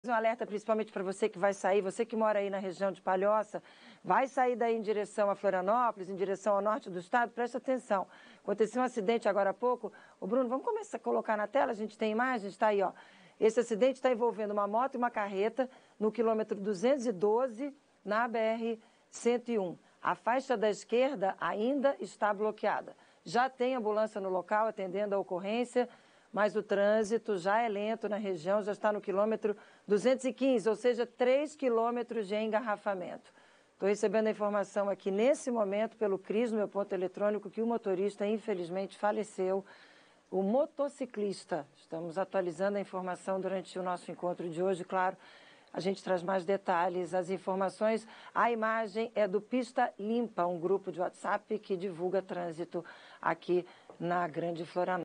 Motociclista morre em acidente na BR-101 e complica trânsito na manhã desta sexta-feira
Acompanhe os primeiros detalhes do acidente que aconteceu no início da manhã desta sexta-feira(5) e deixou o trânsito lento na BR-101, no KM 212, sentido norte do estado.